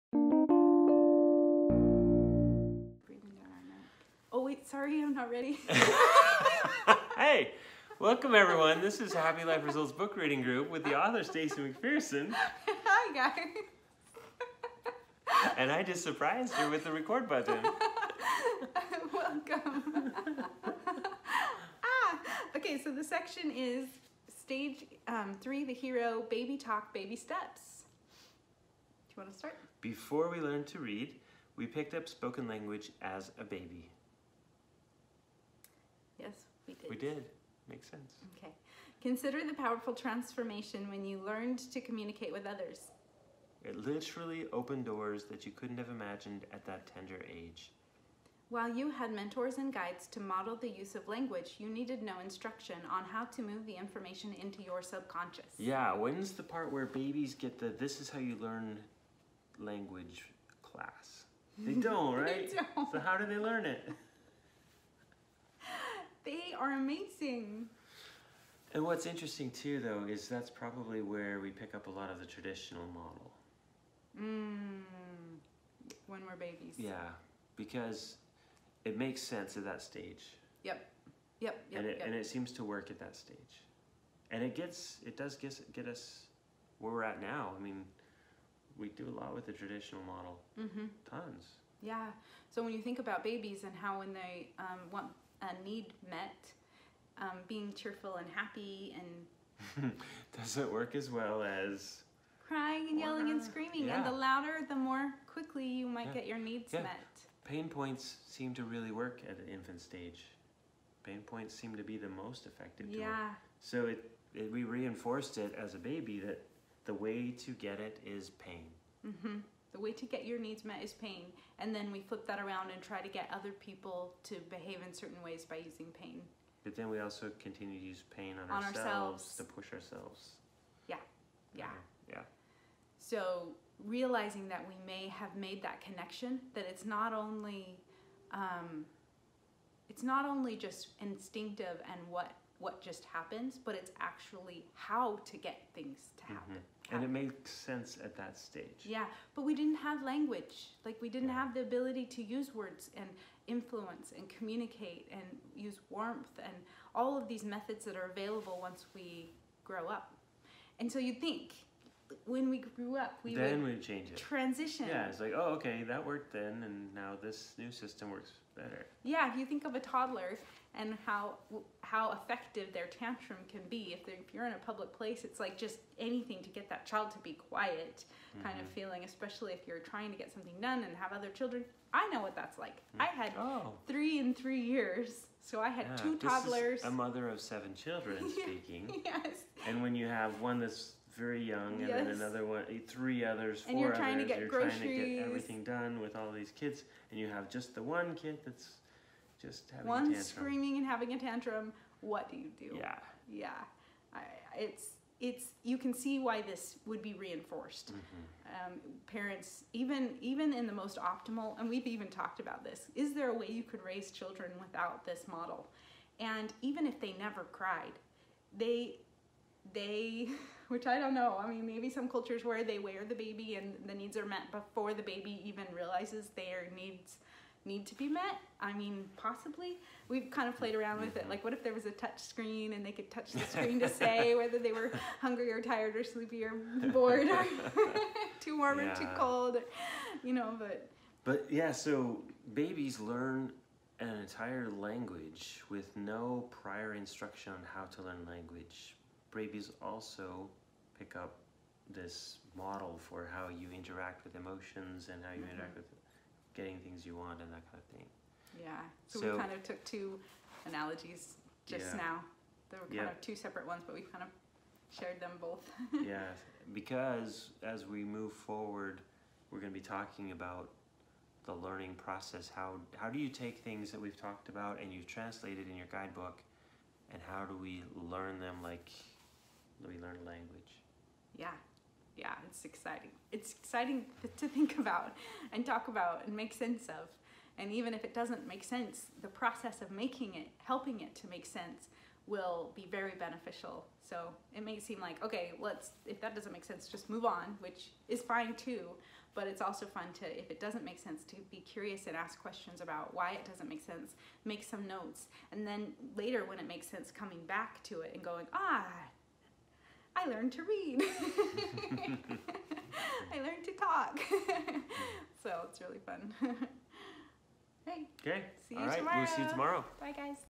Baby Talk, Baby Steps - Day 128
Happy Life Results Book Reading Group

Day 128

Baby Talk, Baby Steps

Before we learned to read, we picked up spoken language as a baby. Consider the powerful transformation when you learned to communicate with others! It literally opened doors that you couldn’t have imagined at that tender age. While you had mentors and guides to model the use of language, you needed no instruction on how to move the information into your subconscious.